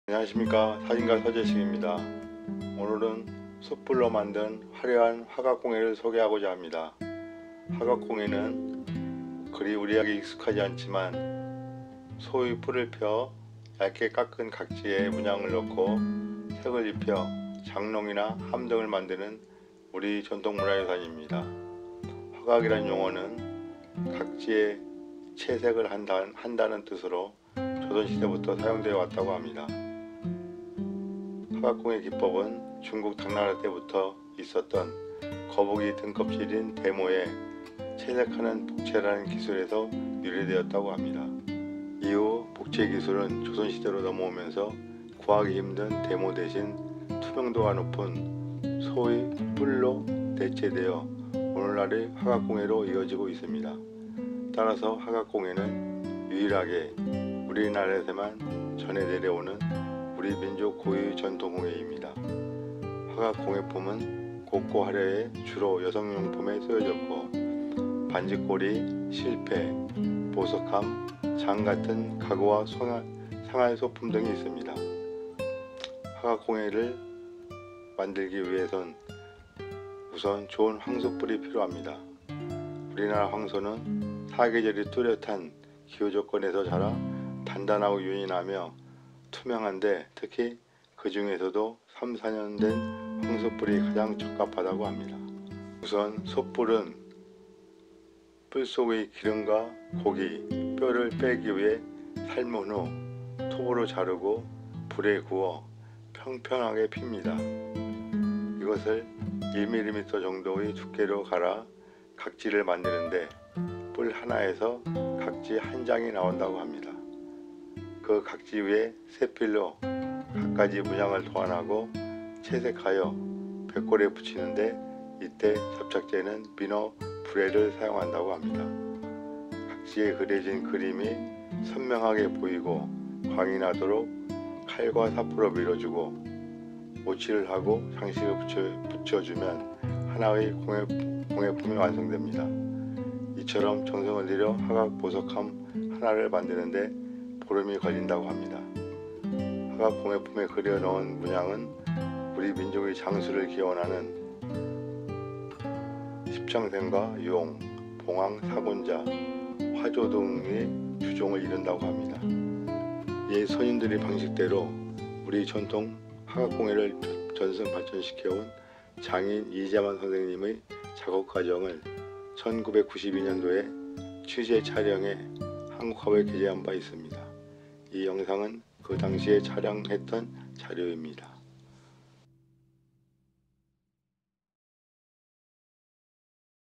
한국의 전통화각공예,한국의전통 장인,화각공예장인 이재만,소뿔로 만든공예,한국의 민속공예,화각장. Traditional Korean crafts, Hwagak crafts.
Hwagak Craft is not very familiar, but it is our traditional cultural heritage that spreads out the horns of a cow, puts patterns in thin cut corners, and colors them to make closets and boxes.
The term Hwagak is said to have been used since the Joseon Dynasty to mean coloring in various places.
The art of painting the painting originated from a technique called Bokchae, which was used since the Tang Dynasty in China to paint the turtle shell, the godmother.

Since then, as the retrofit technology has moved to the Joseon Dynasty, it has been replaced with the horns of a cow with high transparency instead of a godmother, which is difficult to obtain, leading to today's painting craft.
Therefore, Hwagak Craft is the only traditional Korean craft that has been handed down only in Korea.
Hwagak Crafts are fine and colorful, so they are mainly used for women's goods, and there are jewelry boxes in the failure of the jewelry box, furniture such as a market, and household items.
In order to make a painting art, you first need a good bullhorn. Korean bulls grow in different climatic conditions in four seasons and are hard, glossy, and transparent, especially among them, the horns of three to four-year bulls are the most suitable.
The bullhorn is boiled to remove the oil and meat bones from the horn, cut with a saw, and baked over fire to bloom comfortably. This is ground to a thickness of about 1 millimeter to make a horn, and a horn comes out from one horn comes out. For each area, various patterns are designed and colored with a brush and attached to the white bone, and the adhesive is used as a croaker. It takes 15 days to make a painting box with sincerity, such as pushing it with a knife and sandpaper so that the painting can be clearly seen and polished, lacquer and decorations can be applied to completely. The patterns drawn in the Hwagak Crafts are mainly composed of the Tenjangsaengs and Dragon Bonghwang Sagunja Hwajo, which the Korean people have been using to pray for a long life.

We have covered and filmed the work process of Lee Jae-man, a craftsman who has inherited and developed our traditional painting craft in the way of the old ancestors, in 1992 and published it in the Korean pictorial. This video was filmed at that time.